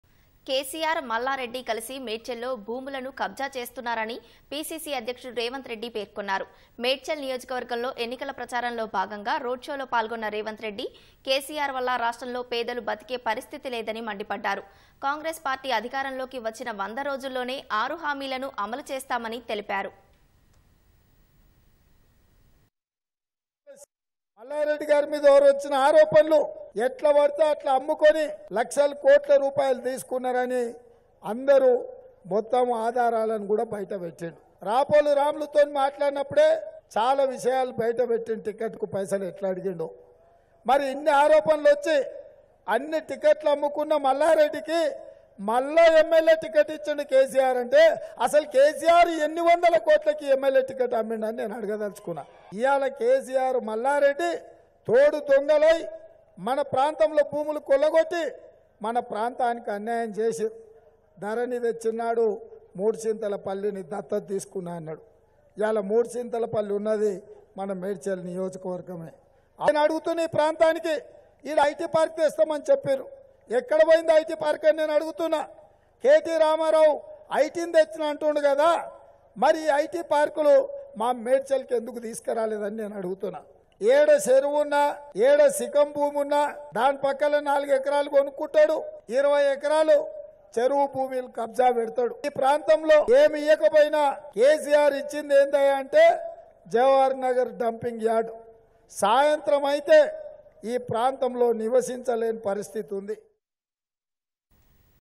KCR, మల్లారెడ్డి కలిసి మేడ్చల్ లో భూములను కబ్జా చేస్తున్నారు | రేవంత్ రెడ్డి
KCR, మల్లారెడ్డి కలిసి మేడ్చల్ లో భూములను కబ్జా చేస్తున్నారని PCC అధ్యక్షుడు రేవంత్ రెడ్డి ఆరోపించారు. మేడ్చల్  నియోజకవర్గంలో ఎన్నికల ప్రచారంలో భాగంగా రోడ్ షోలో పాల్గొన్న రేవంత్ రెడ్డి.. భారాస ప్రభుత్వం వల్ల రాష్ట్రంలో పేదలు బతికే పరిస్థితి లేదని మండిపడ్డారు. కాంగ్రెస్  పార్టీ అధికారంలోకి వచ్చిన 100 రోజుల్లోనే ఆరు హామీలను అమలు చేస్తామని తెలిపారు.